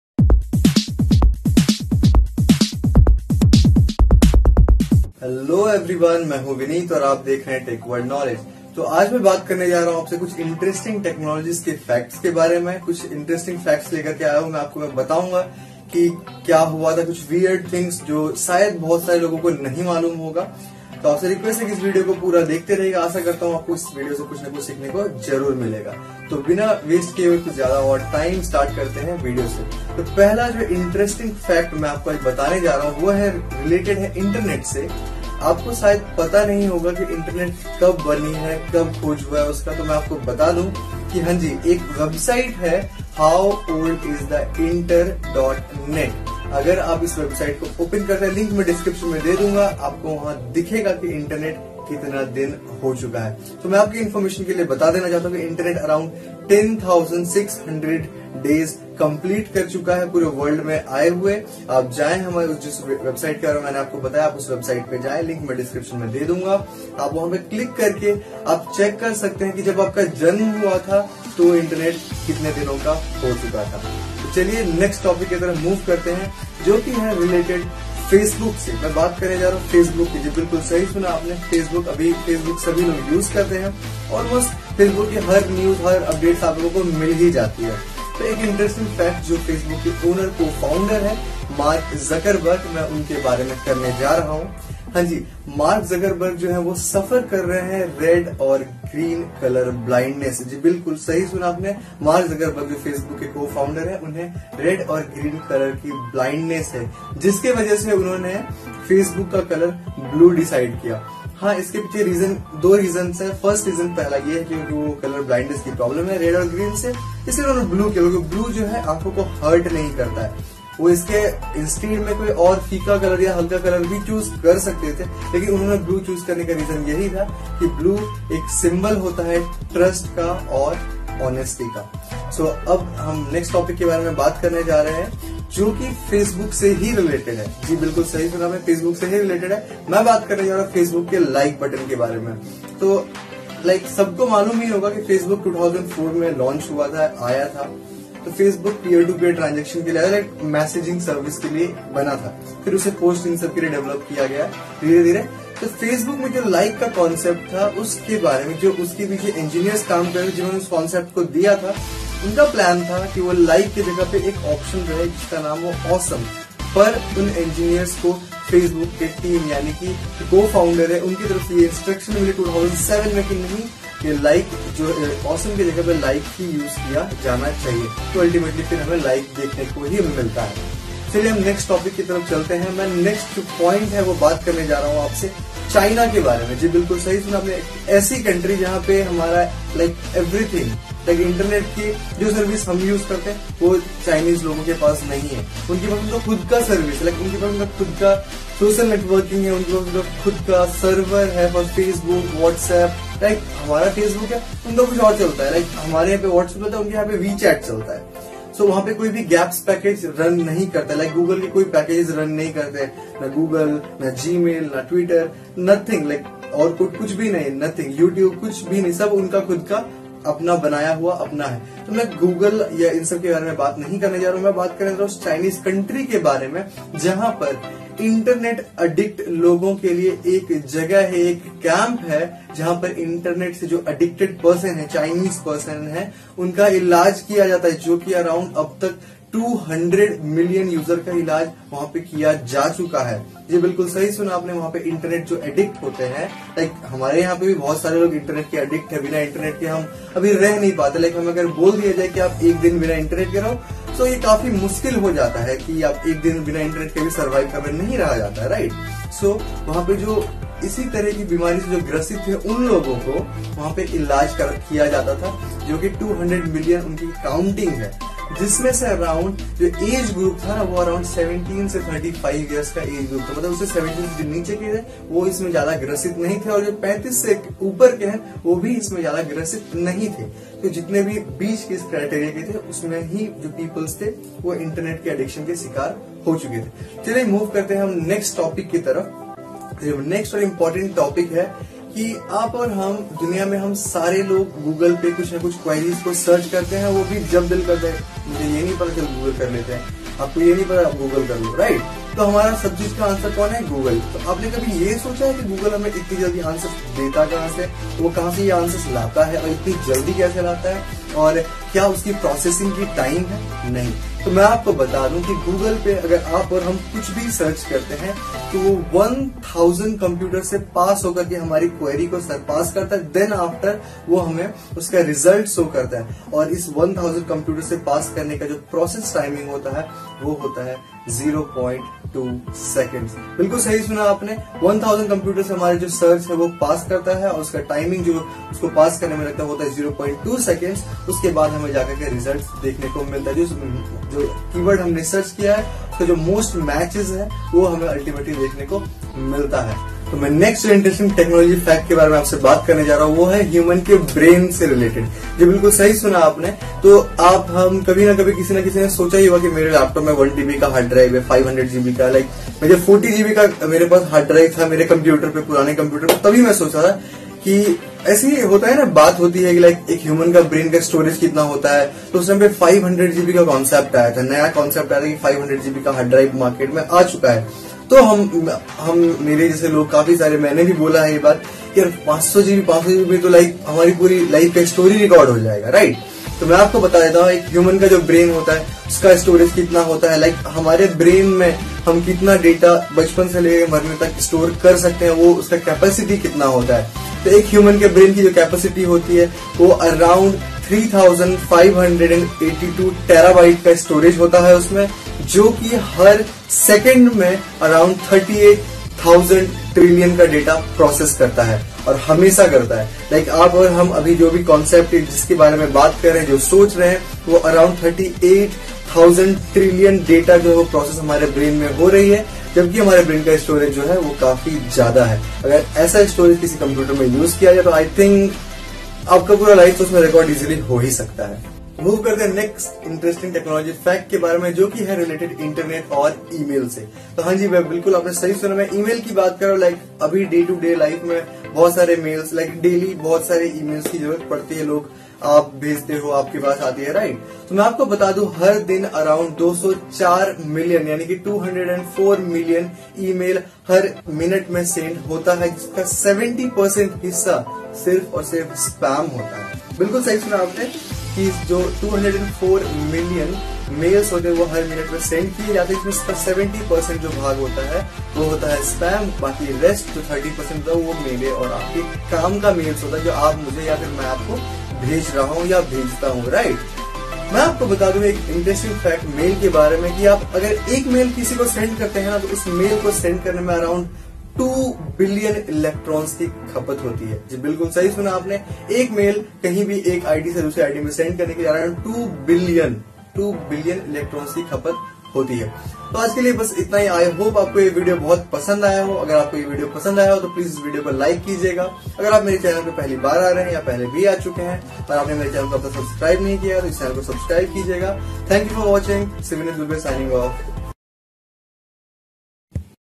Interesting Facts about Internet You Should Know
⦁ Mark Zuckerberg suffers from red-green colorblindness. This is why Facebook’s primary color scheme is blue – although it certainly doesn’t hurt that blue is also strongly associated with trust and security, two concepts essential in getting people to voluntarily part with their personal information
⦁ Facebook’s ‘Like’ button used to be the ‘Awesome’ button. Facebook engineer Andrew Bosworth said that he and other engineers were enthusiastic about the “Awesome” button, but that the idea was ultimately vetoed by Zuckerberg in 2007. The site eventually settled on the “Like” button, a decision that Bosworth said was met with a decidedly lukewarm reception
⦁ 2. If computer’s brain was designed just as a human brain then it could store 3584 terabytes of data and perform 38 thousand trillion operations each second. Do not underestimate your own brain!
⦁ A single Google query uses 1,000 computers in 0.2 seconds to retrieve an answer.
⦁ China has treatment camps for internet addicts. 200 million Internet users in China are between the ages of 15 and 35. Hence, it is most like that they lose self-control. Tao Ran, director of the country’s first Internet addiction treatment clinic under a military hospital in Beijing said that 40% of those addicted to the Internet suffer from attention deficit hyperactivity disorder.
⦁ internet sends approximately 204 million emails per minute and 70% of all the emails sent are spam. 2 billion electrons are required to produce a single email.
⦁ The internet is now 10,000+ days old. If you want to keep an exact track of the counter, you can always visit here and appreciate that you have access to free information anywhere and everywhere.

Best Camera Apps to take DSLR Quality Photos on Android - 2017 ⚡⚡⚡⚡⚡